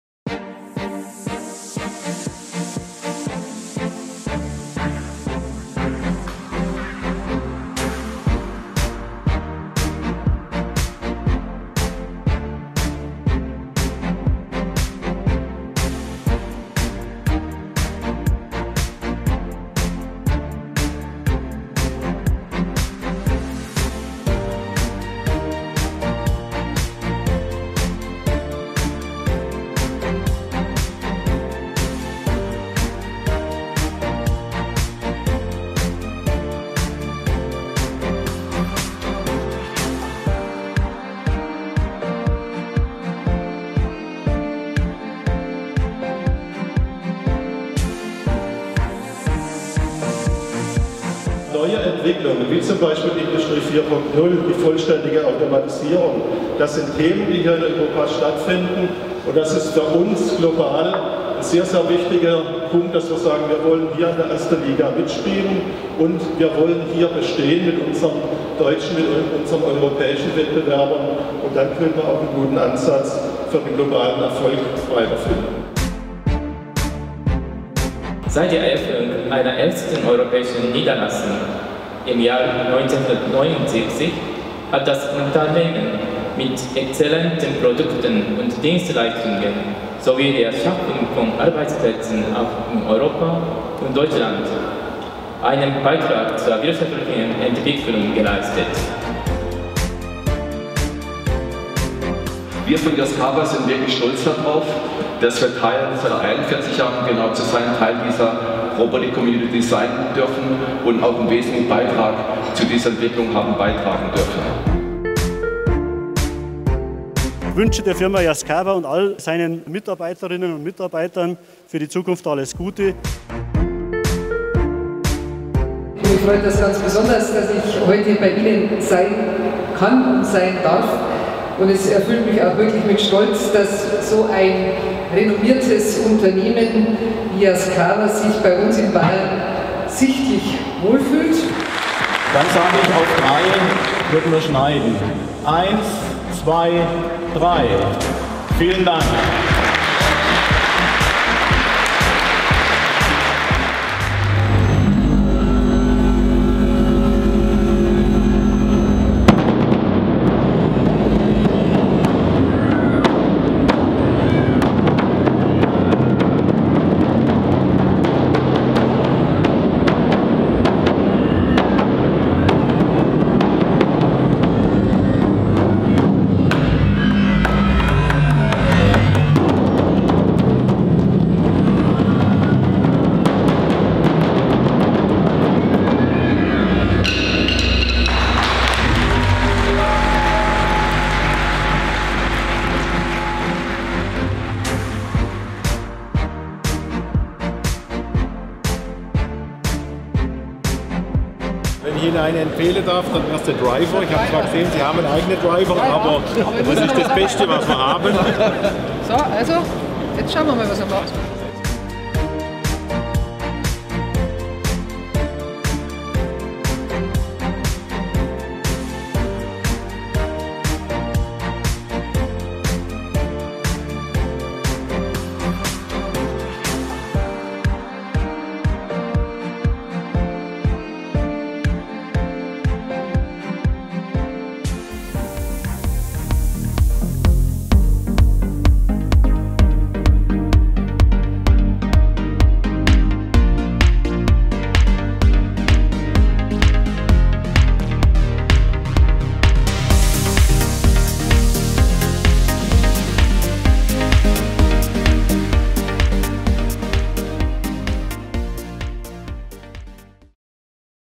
YASKAWA Germany - NEW Opening & Technology Day 2018
Eröffnung des Erweiterungsbaus & Technologie Tag in Allershausen bei München, Januar 2018